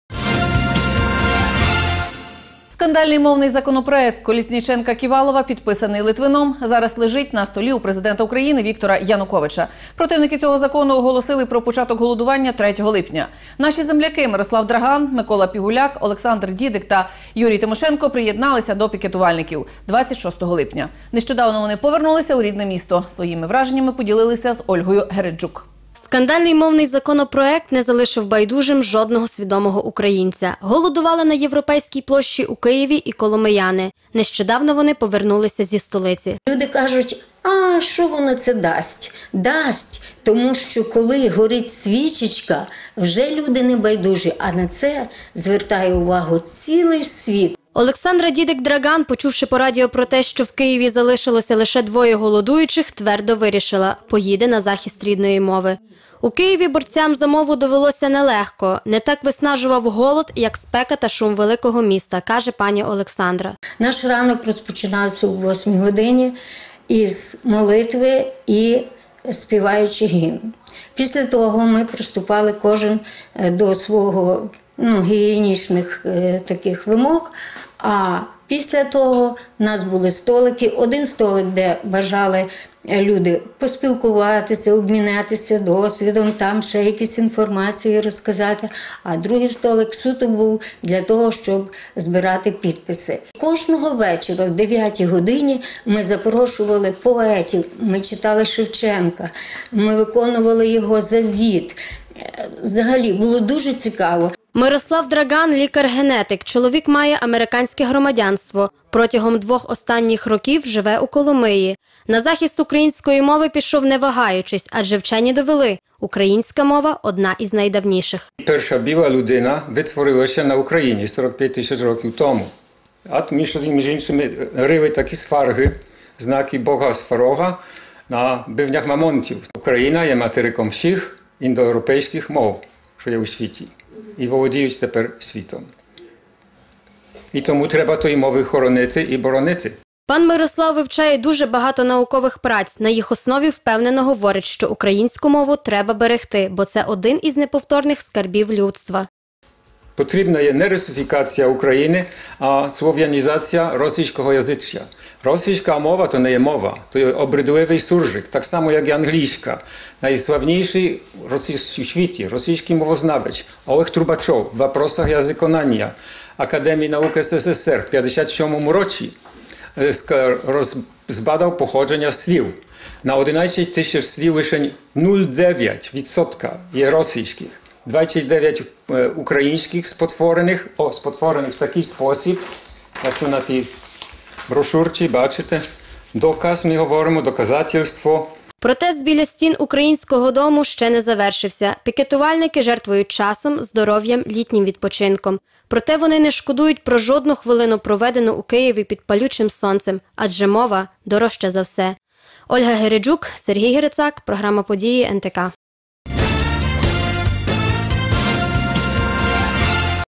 Коломияни, що голодували за мову, повернулися із Києва
Противники цього закону оголосили про початок голодування третього липня. Наші земляки  Мирослав Драґан, Микола Пігуляк, Олександра Дідик та Юрій Тимошенко приєдналися до пікетувальників 26 липня. Нещодавно вони повернулися у рідне місто.